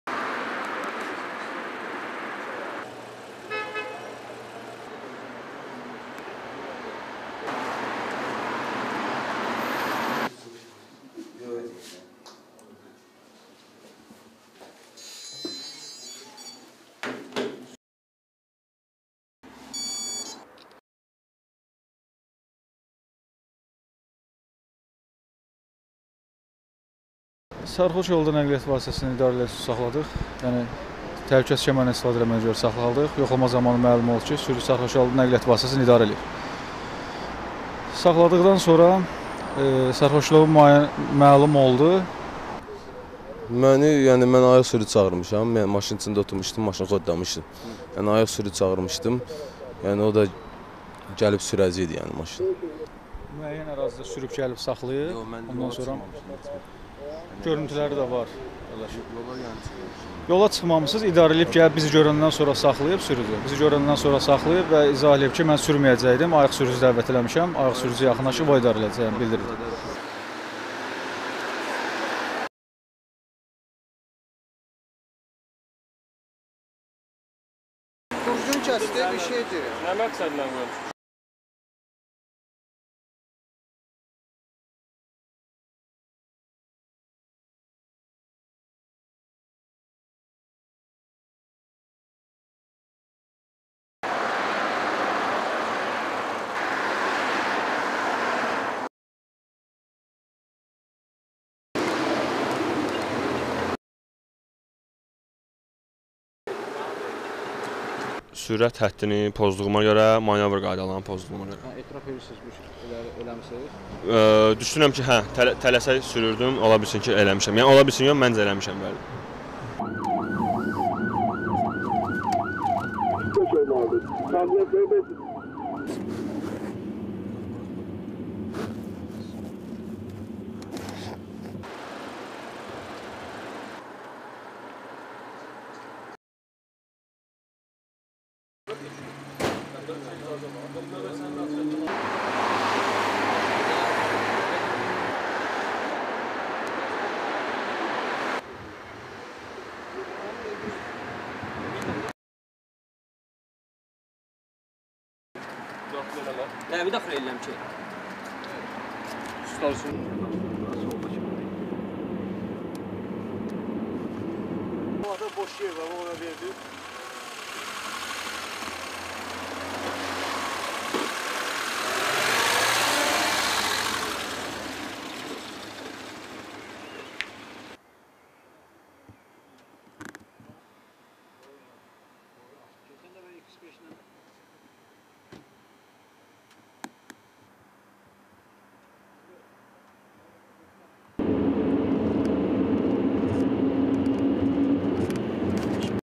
Bakıda yol polisinin gecə reydində görün kimlər tutuldu
Bakı şəhər Baş Polis İdarəsi Dövlət Yol Polisi İdarəsinin əməkdaşları tərəfindən paytaxtın müxtəlif  küçə və prospektlərində gecə saatlarında kobud qayda pozuntuları törədən sürücülərin aşkar edilməsi məqsədi ilə növbəti maarifləndirici-profilaktik tədbir keçirilib.